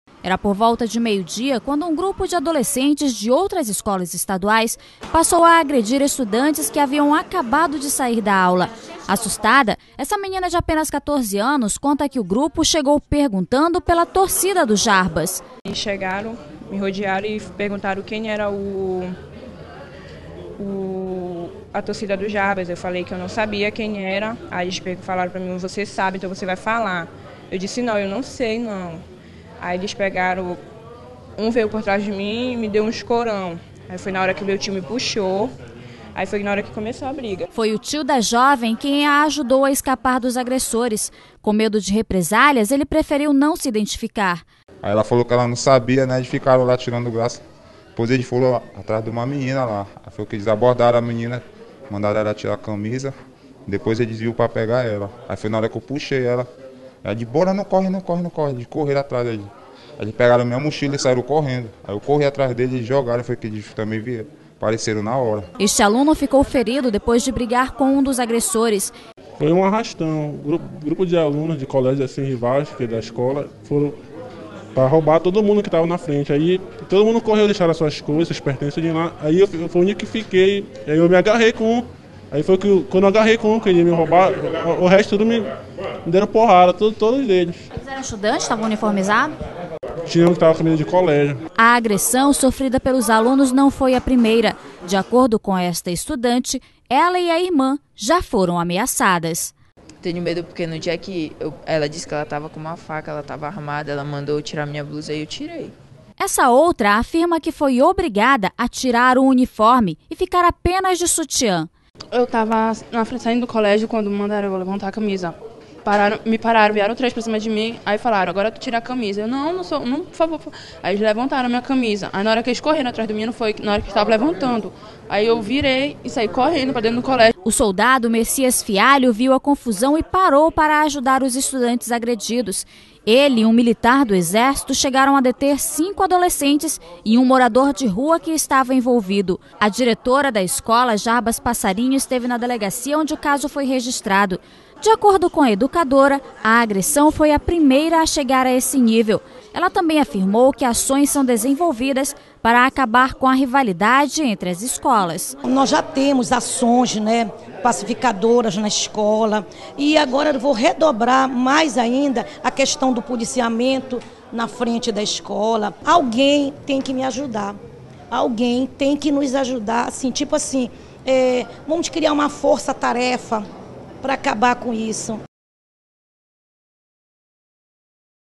Alunos da escola Jarbas Passarinho são agredidos
Estudantes de outras escolas foram os autores das agressões.